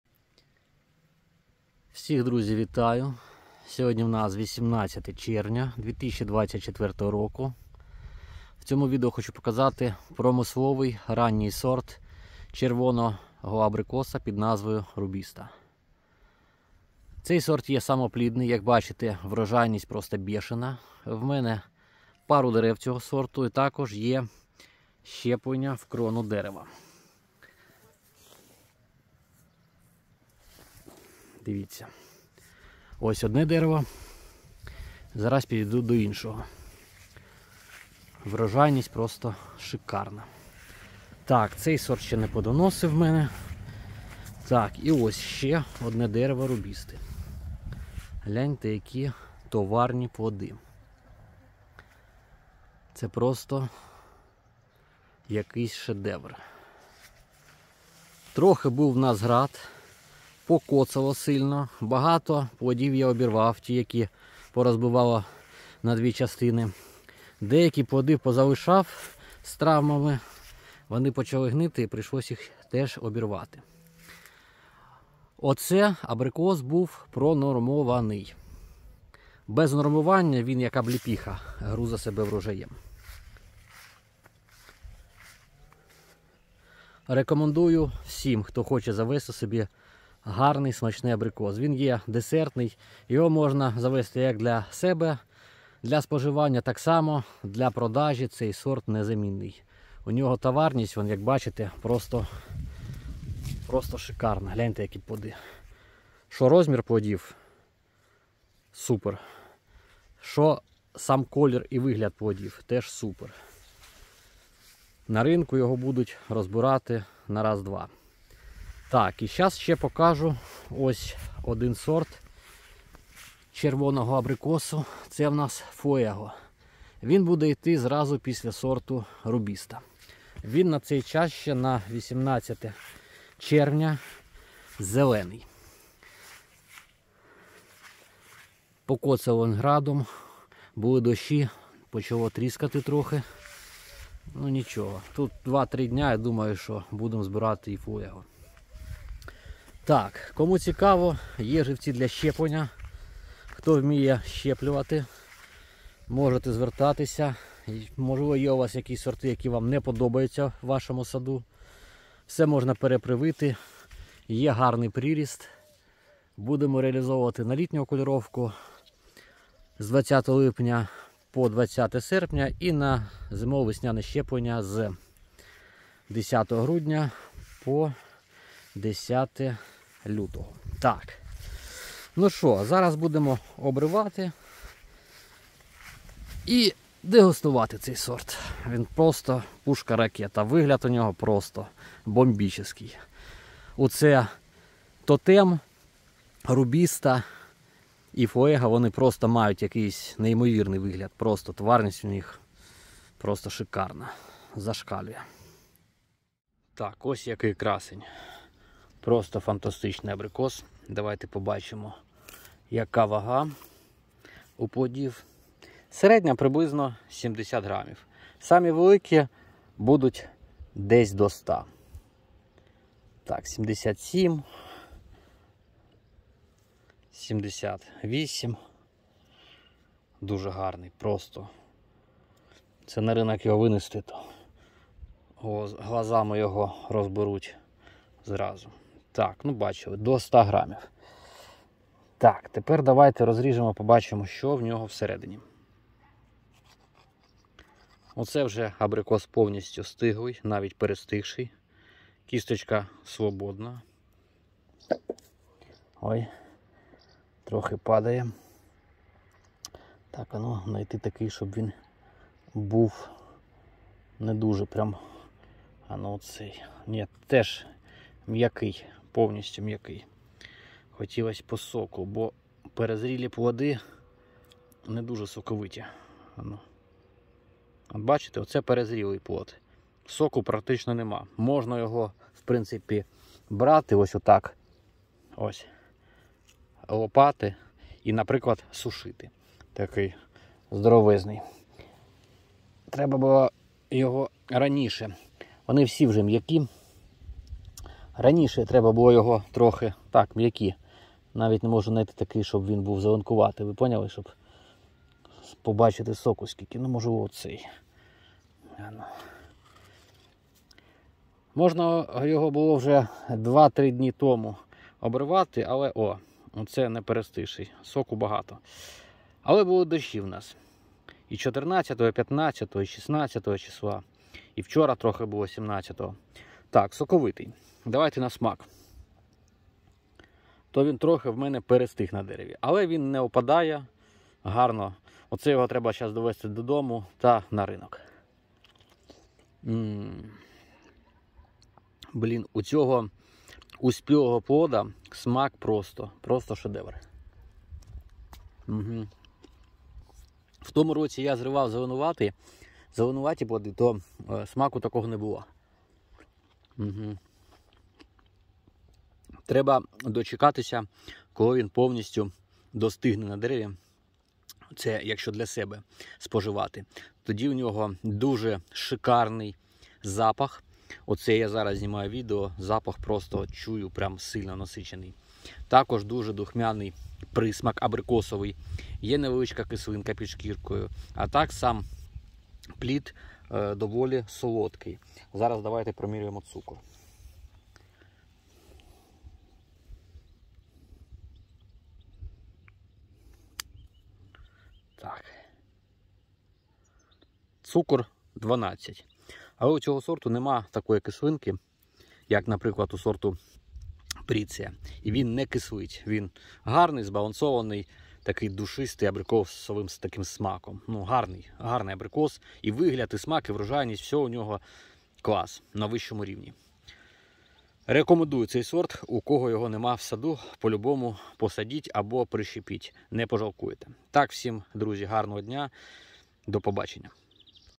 Абрикос Рубіста як завжди красуня.
Абрикос Рубіста товарний ,супер врожайний промисловий сорт який насипає кожен рік яскравими плодами.
 Абрикоса смачна при повному достиганні,смак виражений абрикосовий солодкийз невеликою кислинкою.
Кістка у плодів відокремлюється добре.
Вага плодів в середньому 70-80 грам,окремі 120 грам можуть бути .
Сорт самоплідний і дуже врожайний.
Краща підщепа жердель.
В центральній Україні достигає кінець другої декади червня.
Рекомендую як для власного саду так і для комерції.